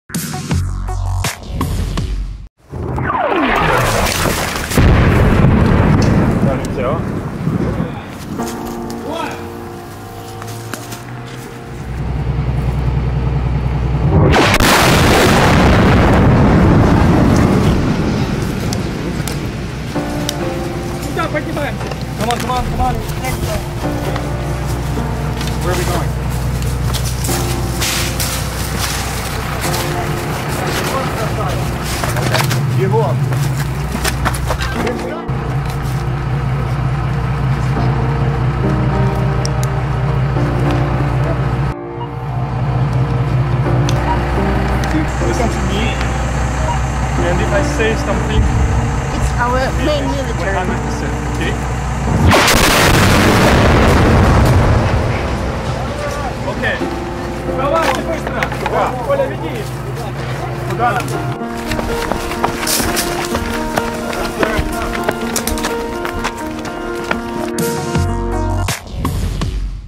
ЛИСИЧАНСК! СРОЧНО! Украинские журналисты попали под обстрел оккупантов в Луганской области
В Лисичанске Луганской области Украинские журналисты попали под обстрел оккупантов.